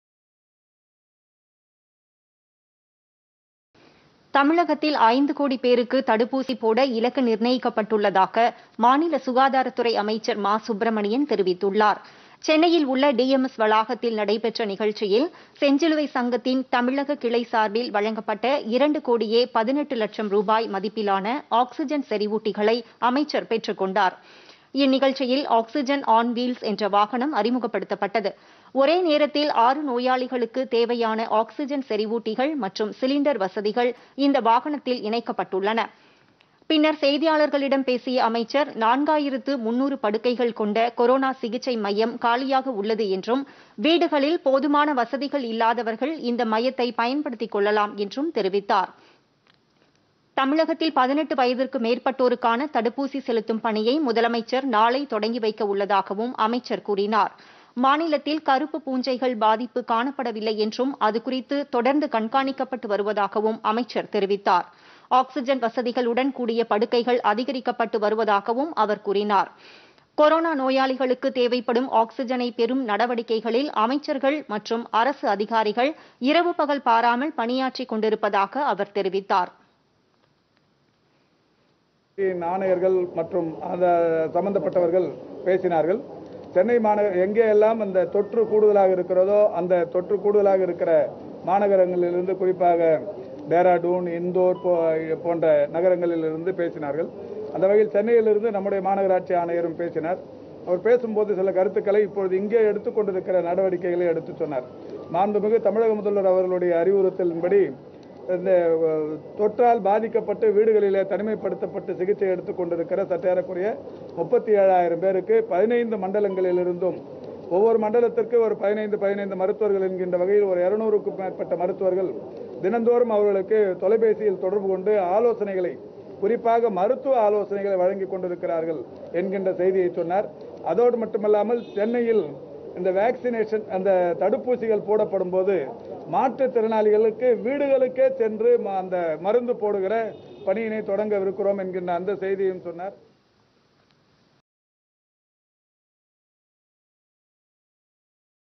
18 வயதுக்கு மேற்பட்டோருக்கு தடுப்பூசிPodhigaiTamilNews
தமிழகத்தில் 18 வயதுக்கு மேற்பட்டோருக்கு தடுப்பூசி போடும் பணி நாளை ஆரம்பம் - மருத்துவம்,  மக்கள் நல்வாழ்வுத்துறை அமைச்சர் மா சுப்பிரமணியன் தகவல்.